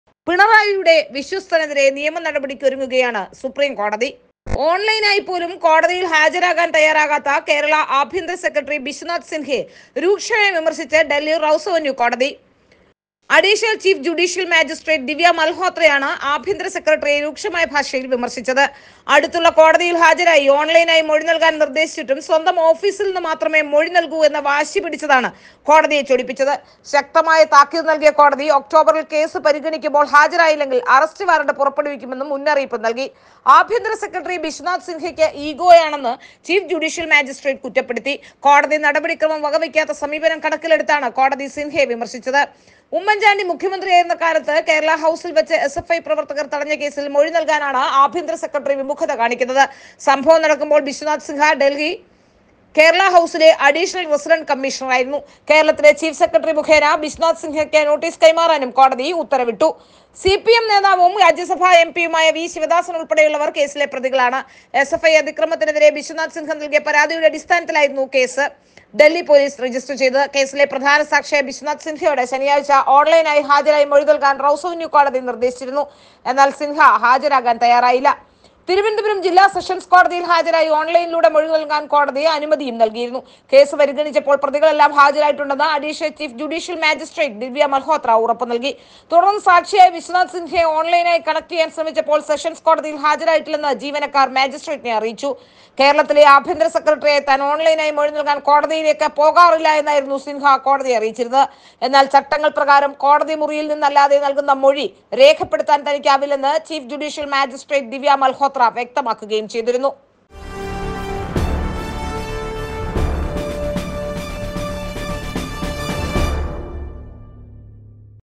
പിണറായിയുടെ വിശ്വസ്തനെ കുടുക്കാൻ സുപ്രീം കോടതി||Bharath Live
a href=" by WangXiNa/a on Freepik